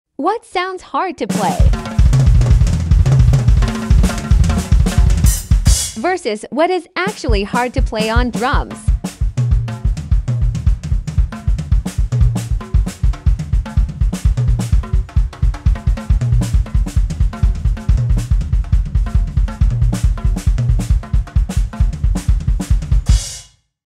What sounds hard vs what is *actually* hard to play on drums
Drums are hard